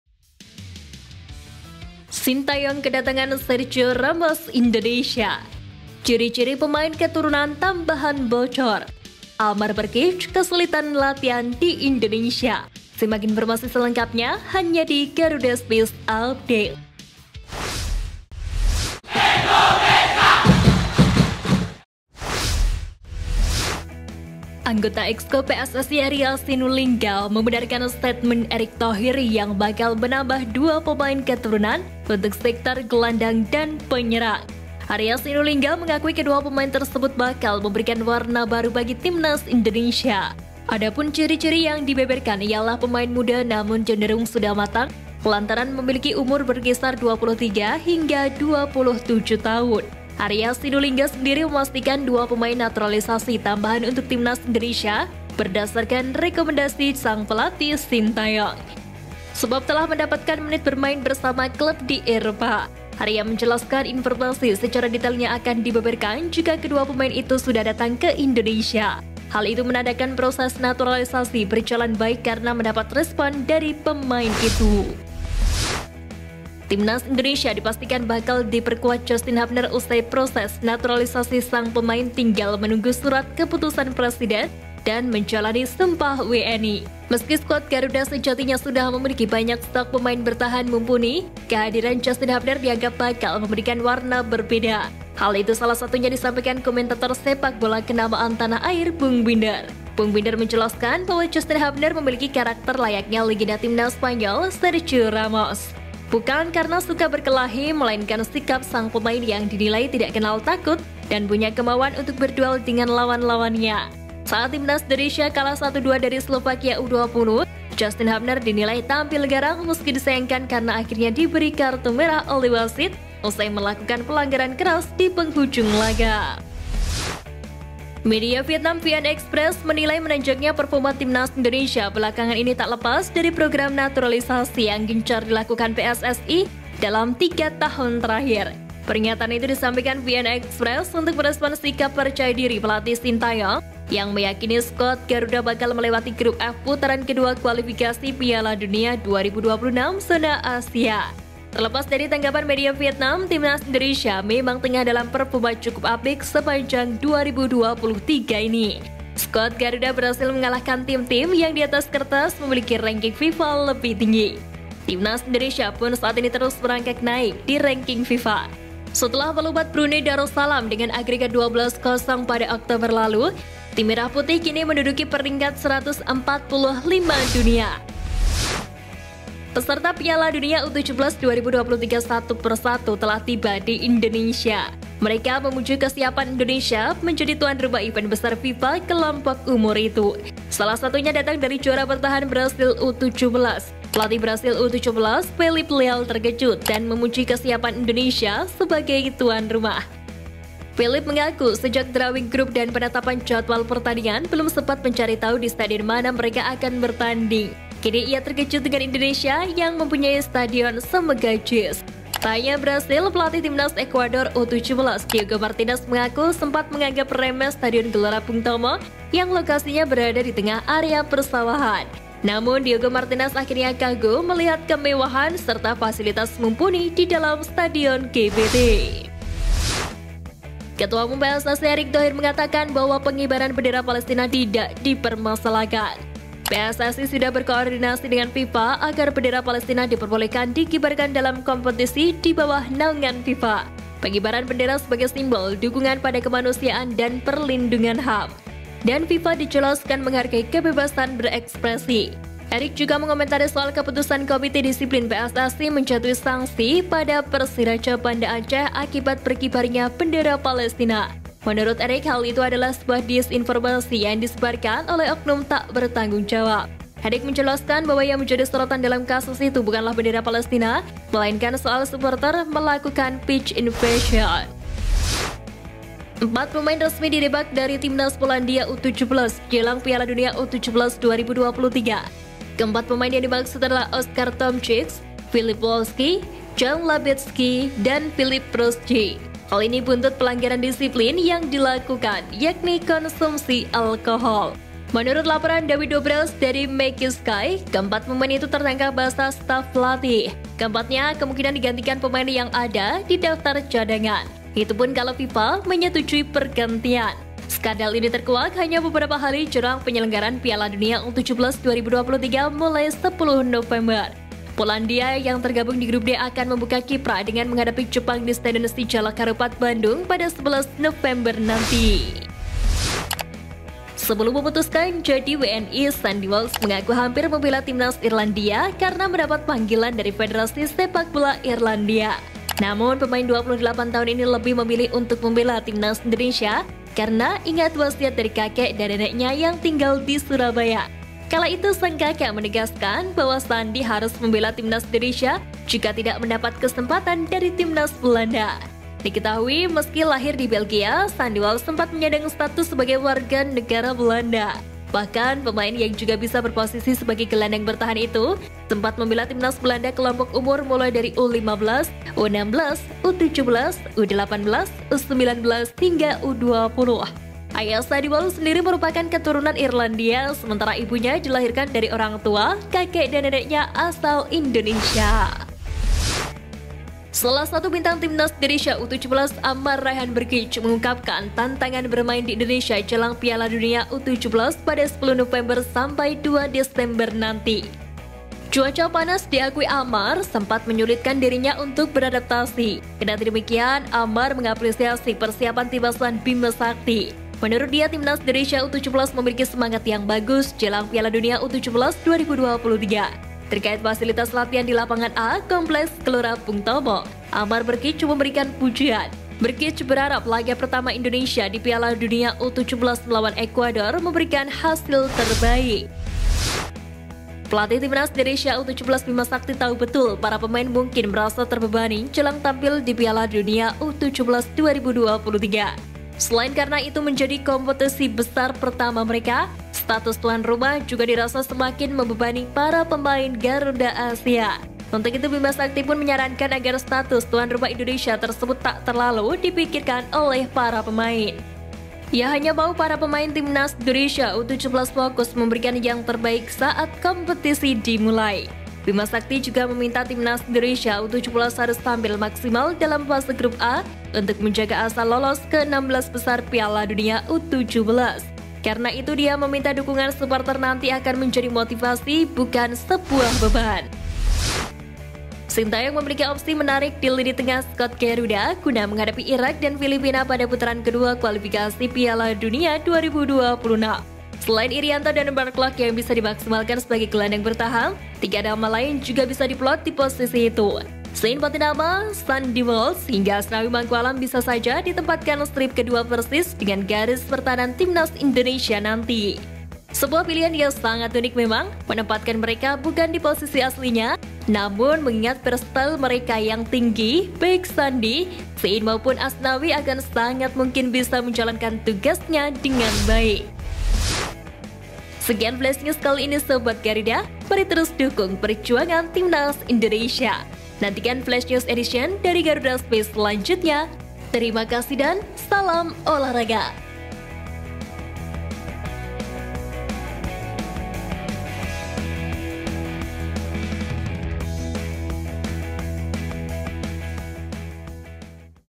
JIS PUKAU BRAZIL U-17! STY PASTIKAN~Sergio Ramos Indonesia DATANG~Ciri2 Pemain Keturunan baru bocor
BERITA BOLA TERBARU DAN TERUPDATE DARI GARUDA SPACE - Berita Bola Hari Selasa, 7 November 2023 : JIS PUKAU BRAZIL U-17! STY PASTIKAN~Sergio Ramos Indonesia DATANG~Ciri2 Pemain Keturunan baru bocor